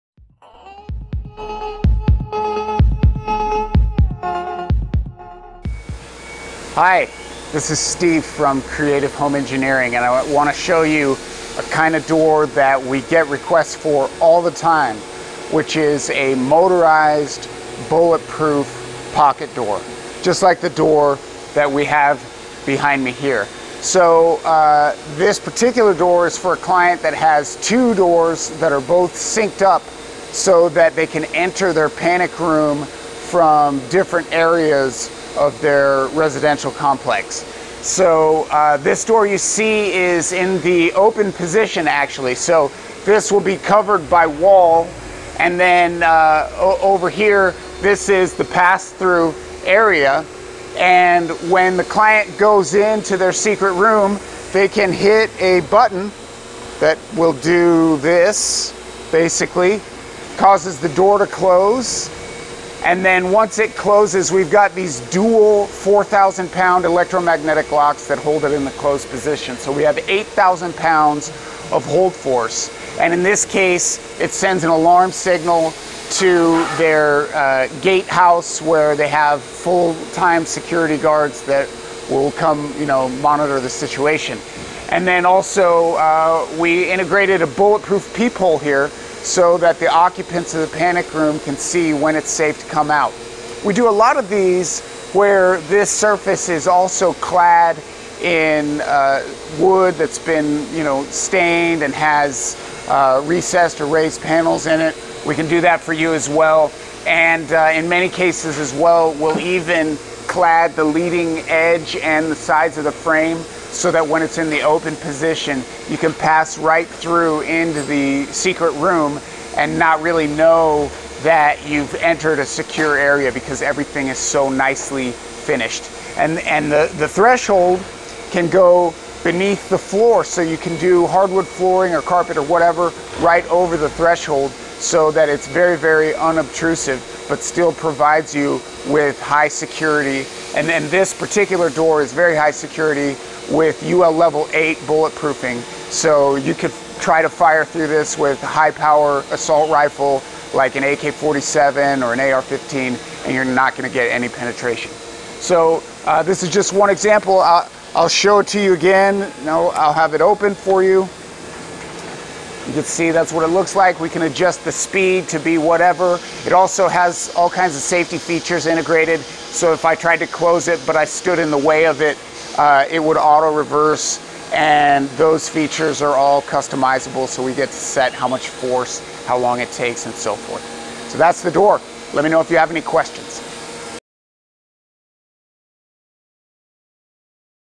A Sliding Bulletproof Pocket Door
This is a type of door that we get a lot of requests for, a bulletproof, motorized, pocket door. Just like all of our custom projects these doors can be made to be completely concealed, even going as far as to cover the threshold! Either way, we guarantee our ability to bring your ideas to life with every custom project we manufacture.

Maybe a more budget friendly option?